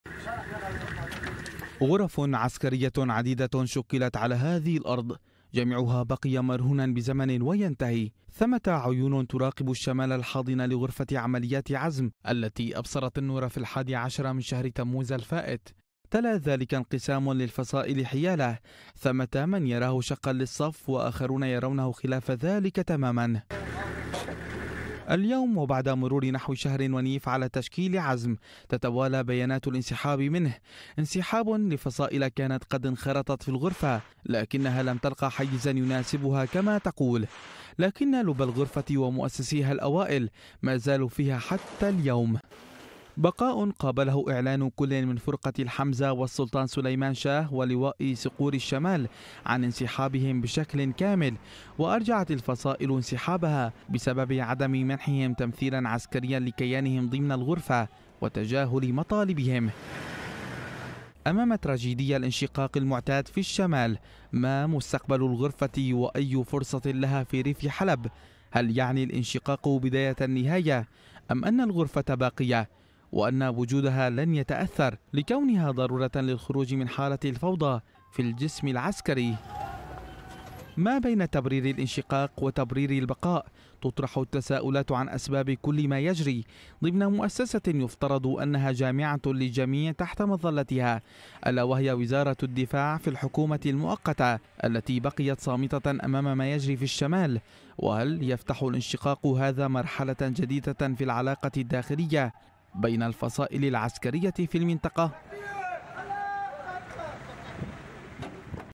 ماذا يجري في عزم.. وما أسباب الانشقاقات داخل فصائلها
أعلنت عدةُ فصائلَ في الجيش الوطني انسحابَها بشكلٍ كامل من غرفةِ عملياتِ عزم المشكلةِ حديثاً، وقالت الفصائلُ إنَّ الانسحابَ جاءَ بسببِ عدمِ منحِهم التمثيلَ العسكريَ لها، مزيد من التفاصيل مع بهاء الحلبي